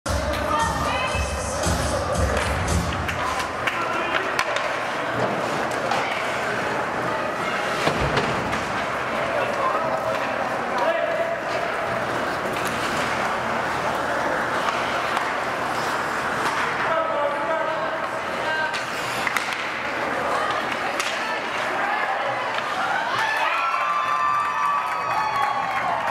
SCS 3 - Jimmy Johns vs SCS - 03/09/22
SCS 3
Jimmy Johns vs SCS - 03/09/22
SCS - 6, Jimmy Johns - 3
Civic Arena, St Clair Shores, MI